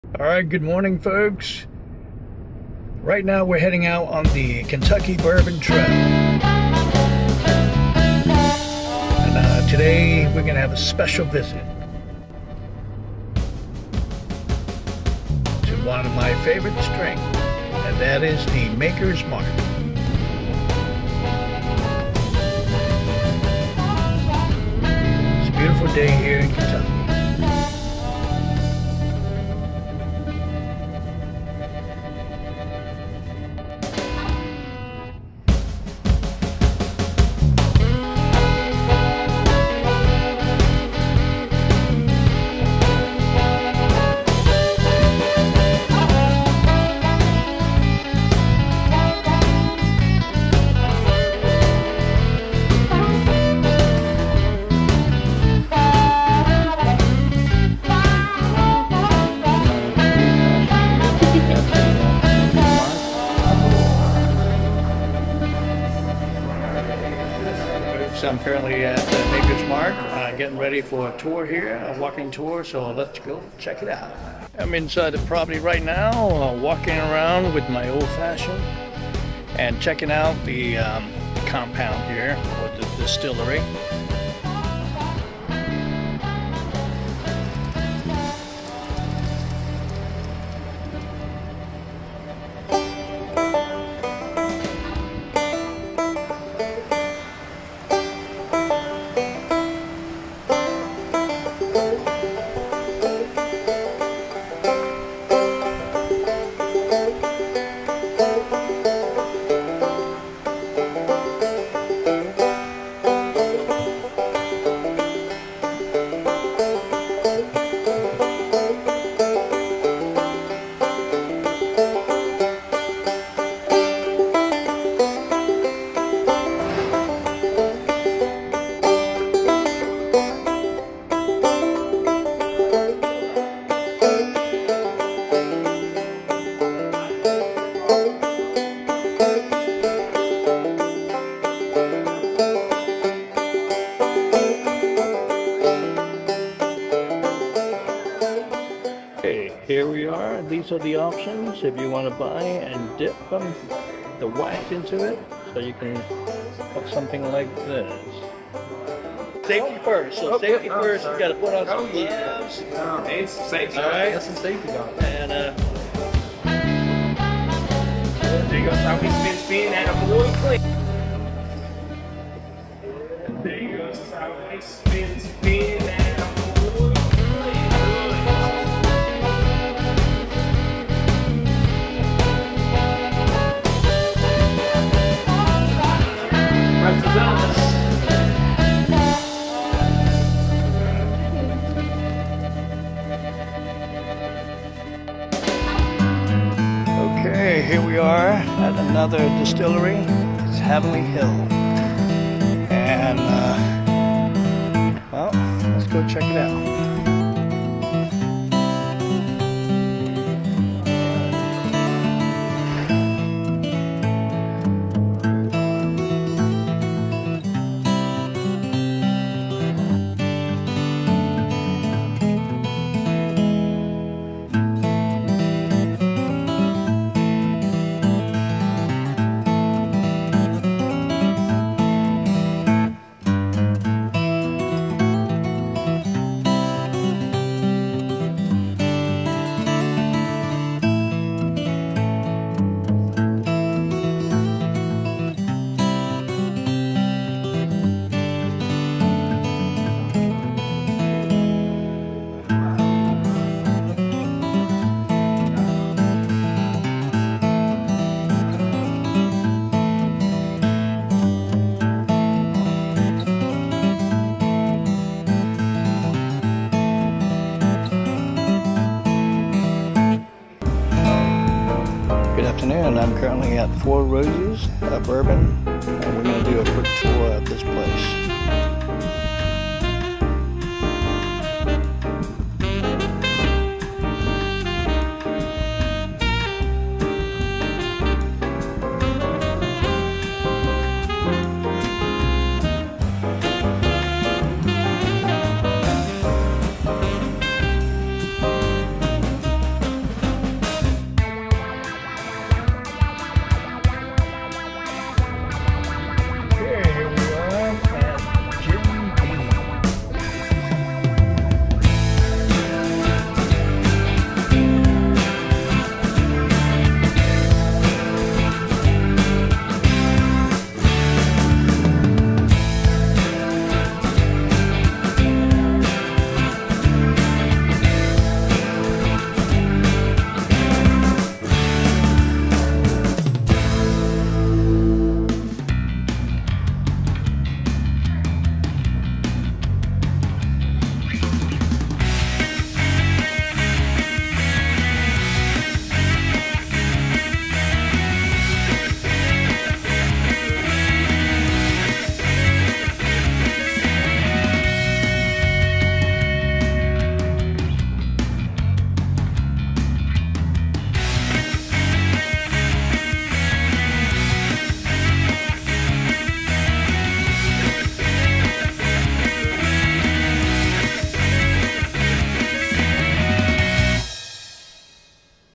Kentucky Bourbon Trail
Touring four distilleries on Kentucky Bourbon Trail:  Maker's Mark, Heavenly Hills, Four Roses, and Jim Beam.  Enjoy the tour.